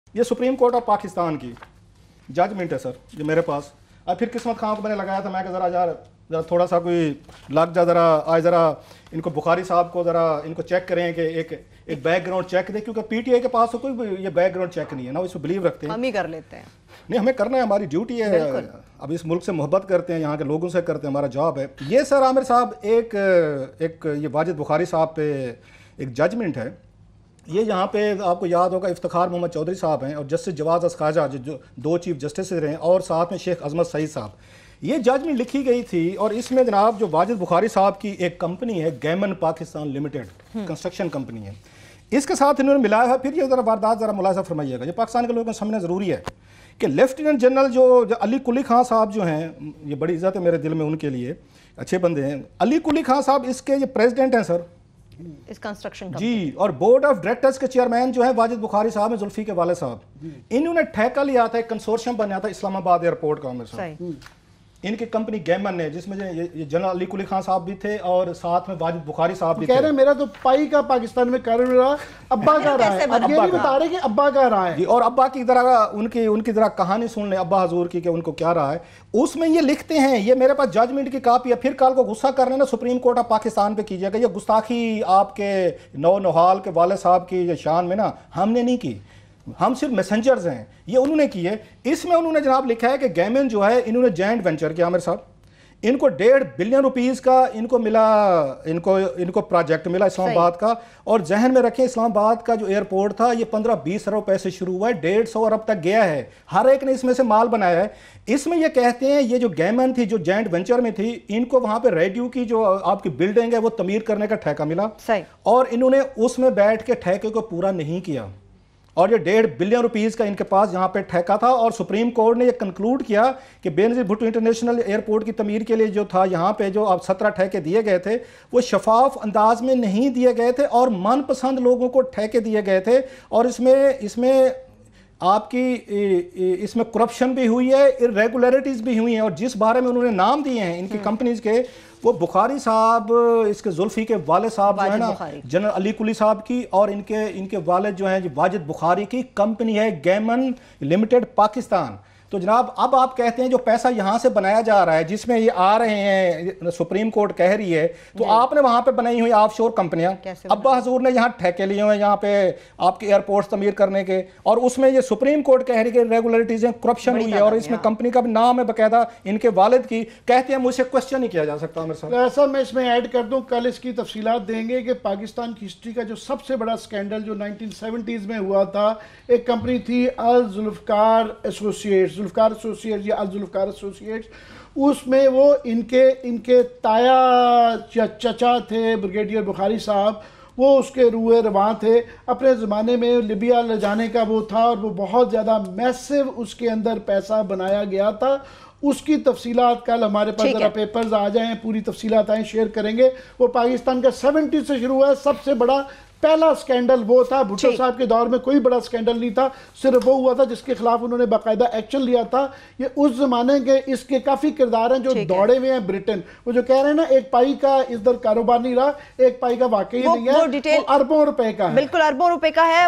Rauf Klasra badly exposed Zulfi Bukhari's father Wajid Bukhari | 12 June 2018 | 92NewsHD
Watch : Rauf Klasra badly exposed Zulfi Bukhari's father Wajid Bukhari | 12 June 2018 | 92NewsHD